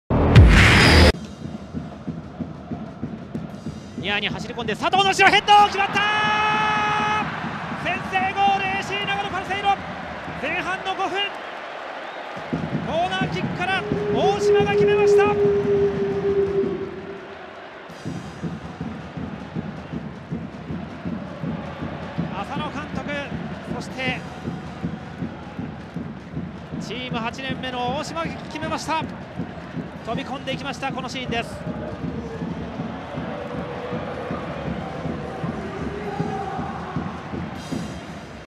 【公式】ゴール動画：大島 嵩弘（長野）5分 ＡＣ長野パルセイロvsＹ．Ｓ．Ｃ．Ｃ．横浜 明治安田生命Ｊ３リーグ 第2節 2017/3/19
左からのコーナーキックに走り込んだ大島 嵩弘（長野）がヘディングゴールを叩き込んで先制点を奪う！ 明治安田Ｊ３のゴール動画を公開中！
2017年3月19日（日）に行われた明治安田生命Ｊ３リーグ 第2節 長野vsYS横浜のゴール動画です。